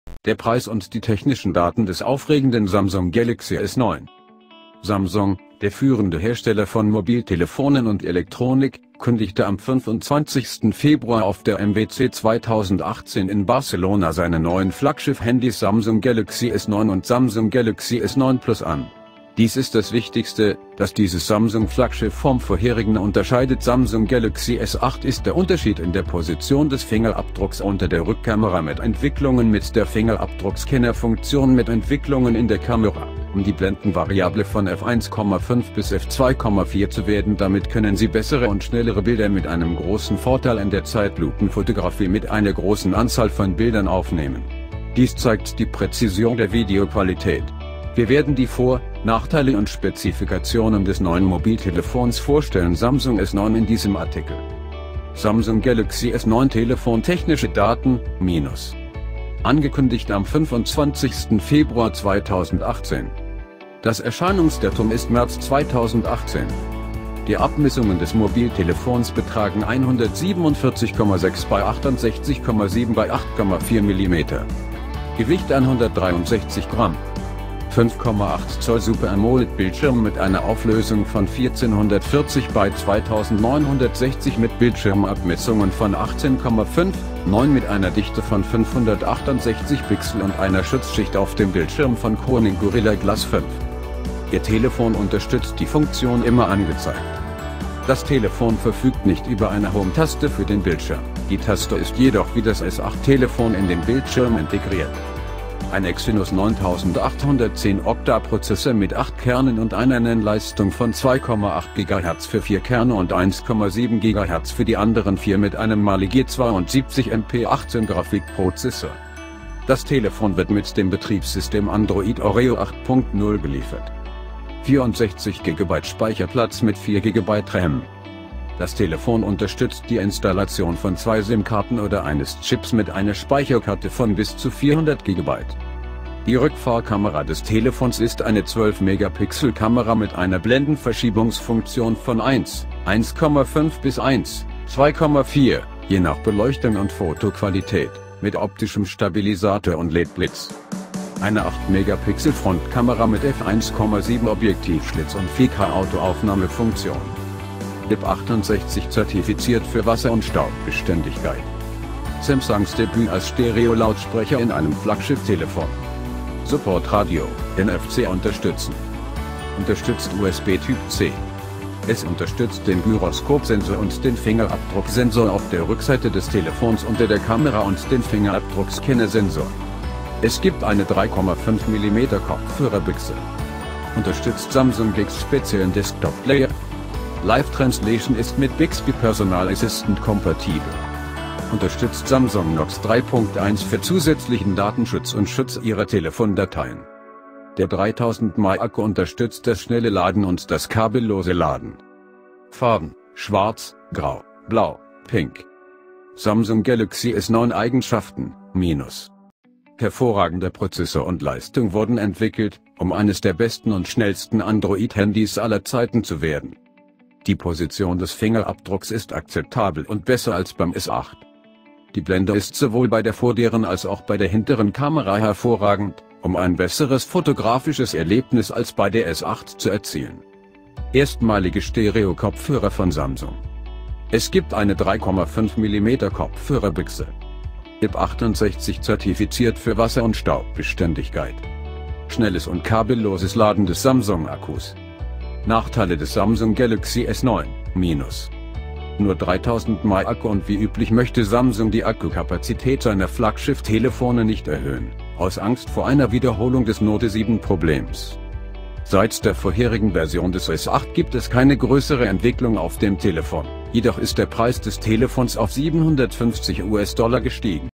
Der Preis und die technischen Daten des aufregenden Samsung Galaxy S9
Samsung, der führende Hersteller von Mobiltelefonen und Elektronik, kündigte am 25. Februar auf der MWC 2018 in Barcelona seine neuen Flaggschiff-Handys Samsung Galaxy S9 und Samsung Galaxy S9 Plus an. Dies ist das Wichtigste, das dieses Samsung-Flaggschiff vom vorherigen unterscheidet Samsung Galaxy S8 ist der Unterschied in der Position des Fingerabdrucks unter der Rückkamera mit Entwicklungen mit der Fingerabdruckscanner-Funktion mit Entwicklungen in der Kamera, um die Blendenvariable von F / 1,5 bis F / 2,4 zu werden Damit können Sie bessere und schnellere Bilder mit einem großen Vorteil in der Zeitlupenfotografie mit einer großen Anzahl von Bildern aufnehmen. Dies zeigt die Präzision der Videoqualität. Wir werden die Vor-, Nachteile und Spezifikationen des neuen Mobiltelefons vorstellen Samsung S9 in diesem Artikel.

Samsung Galaxy S9 Telefon Technische Daten: -
Angekündigt am 25. Februar 2018.
Das Erscheinungsdatum ist März 2018.
Die Abmessungen des Mobiltelefons betragen 147,6 x 68,7 x 8,4 mm.
Gewicht 163 Gramm.
5,8-Zoll-Super-AMOLED-Bildschirm mit einer Auflösung von 1440 x 2960 mit Bildschirmabmessungen von 18,5: 9 mit einer Dichte von 568 Pixel und einer Schutzschicht auf dem Bildschirm von Corning Gorilla Glass 5.
Ihr Telefon unterstützt die Funktion Immer angezeigt.
Das Telefon verfügt nicht über eine Home-Taste für den Bildschirm, die Taste ist jedoch wie das S8-Telefon in den Bildschirm integriert.
Ein Exynos 9810 Octa-Prozessor mit acht Kernen und einer Nennleistung von 2,8 GHz für 4 Kerne und 1,7 GHz für die anderen 4 mit einem Mali-G72 MP18-Grafikprozessor.
Das Telefon wird mit dem Betriebssystem Android Oreo 8.0 geliefert.
64 GB Speicherplatz mit 4 GB RAM.
Das Telefon unterstützt die Installation von zwei SIM-Karten oder eines Chips mit einer Speicherkarte von bis zu 400 GB.
Die Rückfahrkamera des Telefons ist eine 12-Megapixel-Kamera mit einer Blendenverschiebungsfunktion von 1: 1,5 bis 1: 2,4, je nach Beleuchtung und Fotoqualität, mit optischem Stabilisator und LED-Blitz.
Eine 8-Megapixel-Frontkamera mit F / 1,7-Objektivschlitz und 4K-Auto-Aufnahmefunktionen.
IP68-zertifiziert für Wasser- und Staubbeständigkeit.
Samsungs Debüt als Stereolautsprecher in einem Flaggschiff-Telefon.
Support Radio, NFC unterstützen.